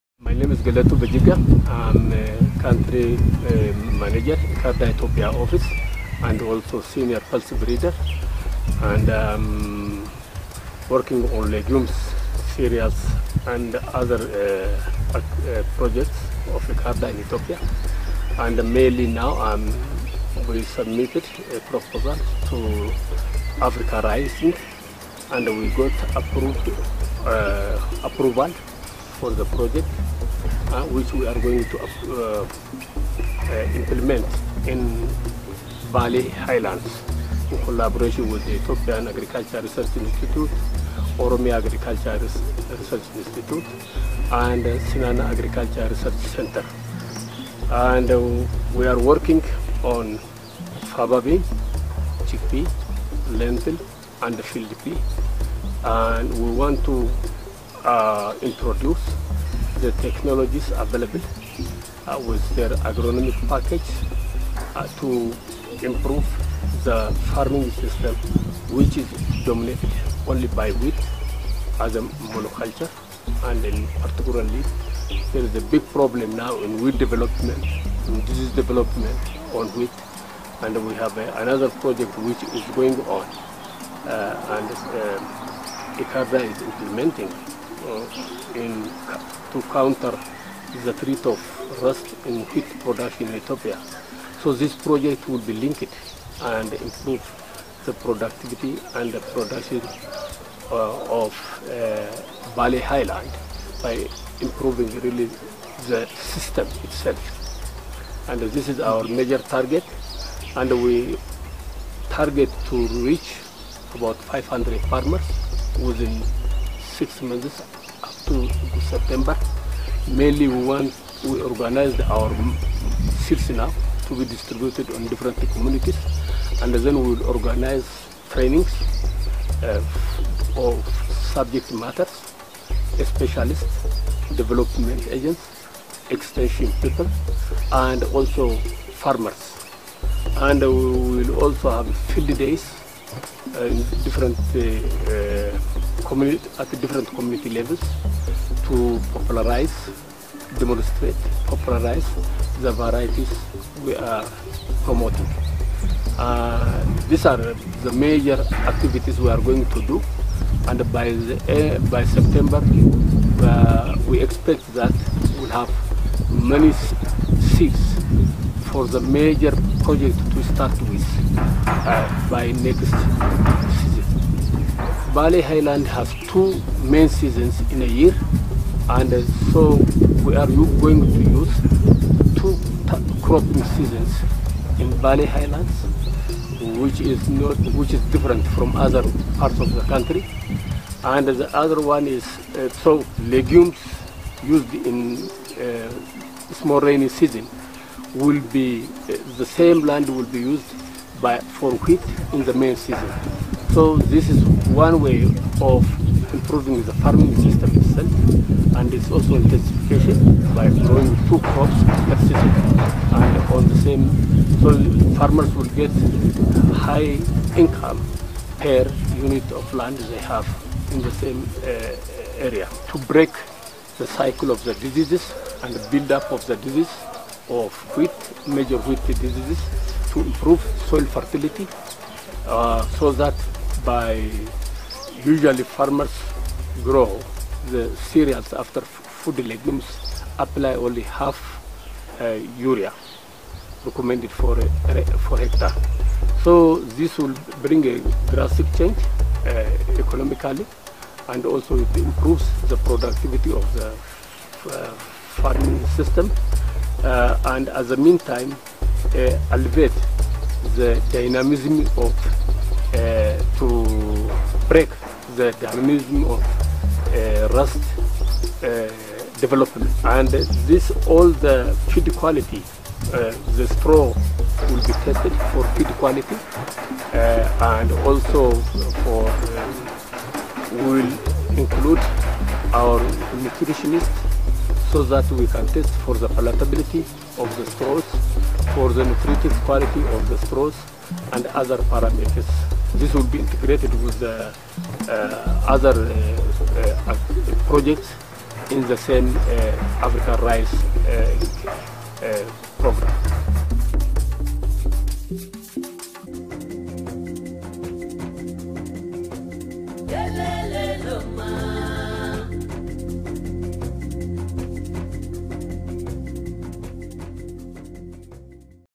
Africa-RISING early win project -- Pulse crops as a pathway to increase crop-livestock productivity
The Africa RISING program comprises three linked research-for-development projects, funded by the USAID Feed the Future Initiative. To produce some short-term outputs and to support the longer term objectives of the projects, in 2012 Africa RISING funded several small, short-term projects in each of the regions. Here Geletu Bejiga introduces an ICARDA 'early win' project that aims to improve the productivity of crop-livestock production systems in Ethiopia by integrating pulse crops into systems currently dominated by wheat production